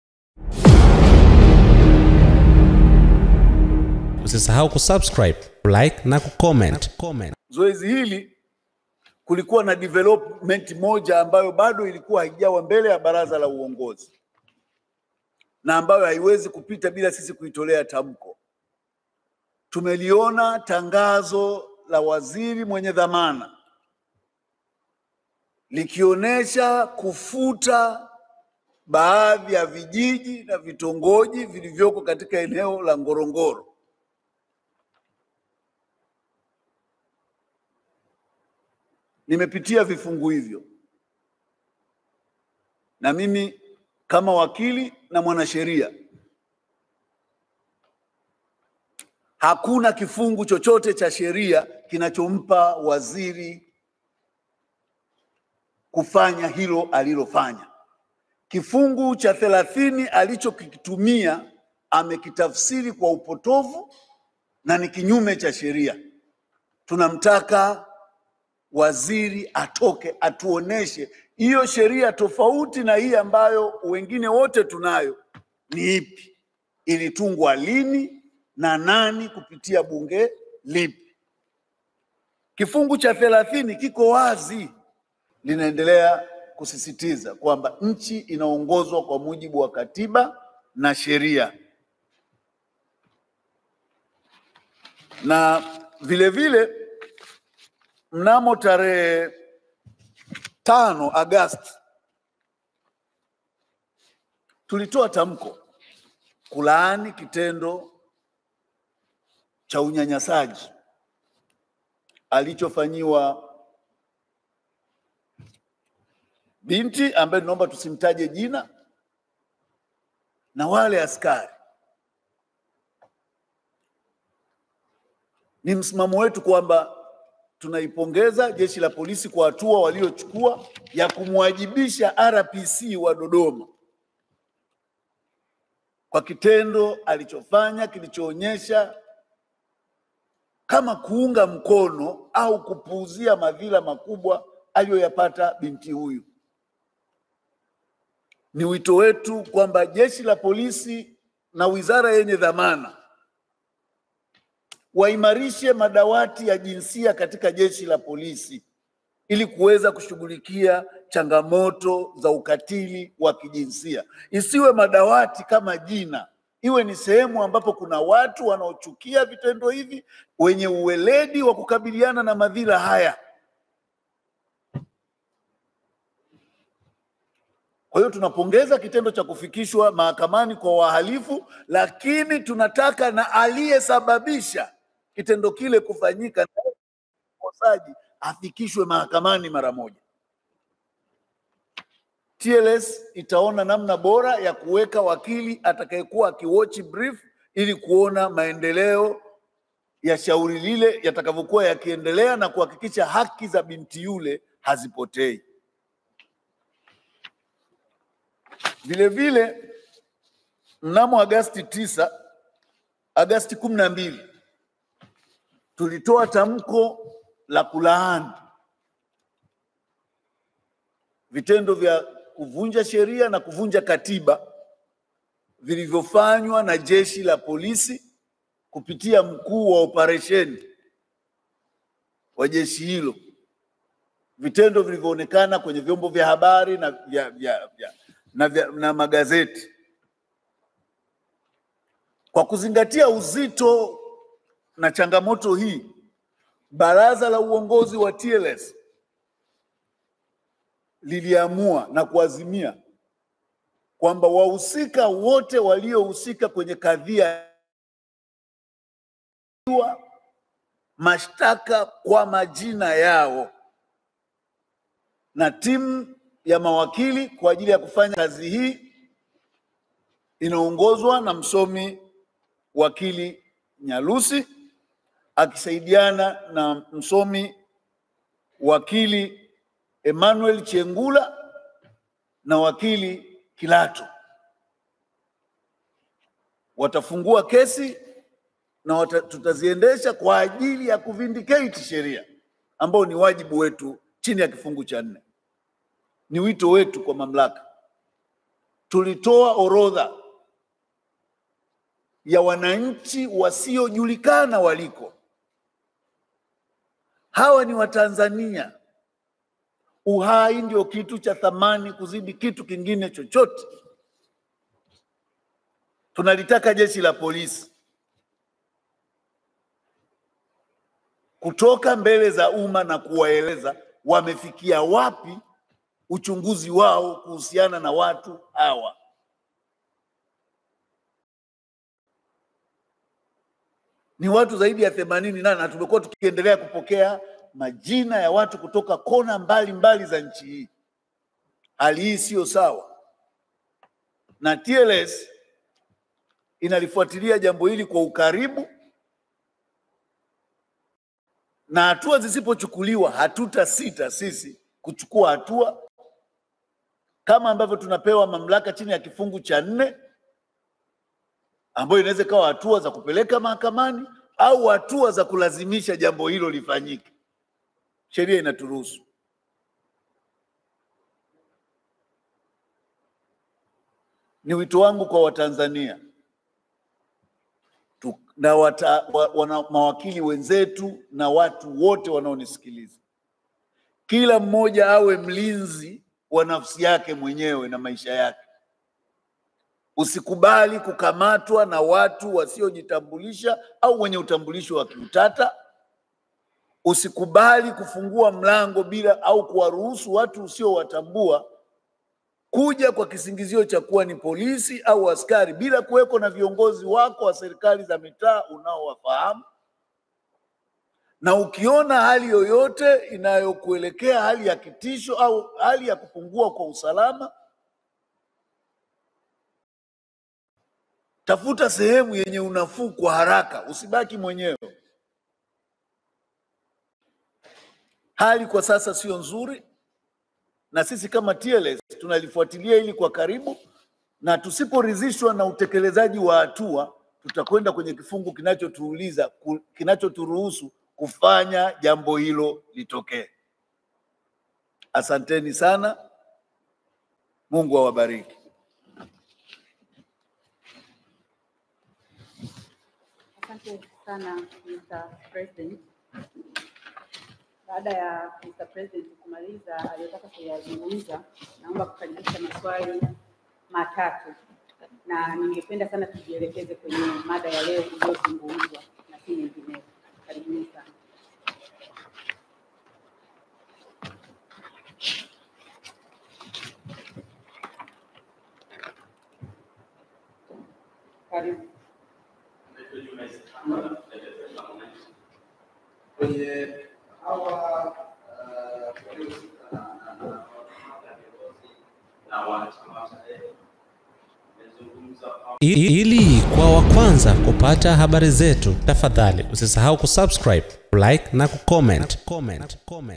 MWABUKUSI AWANG'ANG'ANIA POLISI/ ATAKA 'AFANDE' ALIYESABABISHA BINTI KULAWITIWA AKAMATWE HARAKA
RAIS wa Chama cha Mawakili Tanganyika (TLS), Boniface Mwabukusi amelipongeza jeshi la polisi kwa kuwafikisha mahakamani watuhumiwa wanne wanaodaiwa kubaka na kulawiti binti mmoja aliyepewa jina la XY huku pia akilitaka jeshi hilo kuhakikisha wanamfikisha mahakamani aliyeagiza vijana hao kutenda ukatili huo wa kingono.